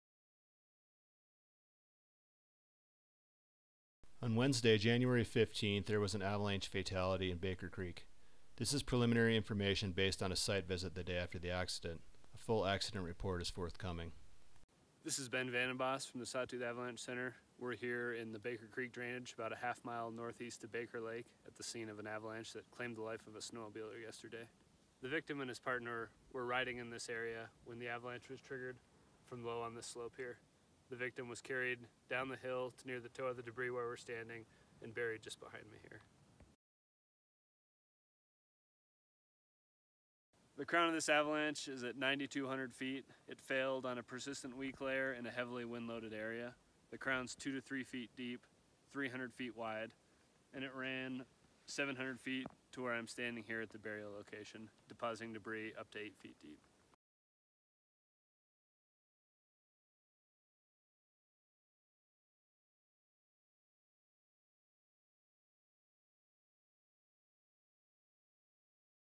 Baker Creek Accident Site Investigation - Galena Summit & Eastern Mtns Zone - January 16th, 2020
On January 15th, 2020, a snowmobiler was caught and killed in an avalanche in the Baker Creek drainage, approximately a half-mile northeast of Baker Lake. The next day, SAC forecasters visited the site to collect data on the avalanche. This video contains preliminary information collected during the site investigation; a full accident report will be published soon.

The avalanche released at an elevation of 9200 feet on a south-facing, wind-loaded slope. The slab broke 2-3 feet deep, 250-300 feet wide, and ran just over 600 vertical feet. The slope angle of the start zone was around 33 degrees, but the slope steepened significantly below the fracture line. The avalanche was triggered from lower on the slope, well below the crown line of the avalanche, and released on a persistent weak layer of faceted snow in the middle to lower snowpack. The avalanche debris range from 3-8 feet deep.

A full accident report will be published once interviews with witnesses are complete.

We extend our heartfelt condolences to the victim’s friends and family.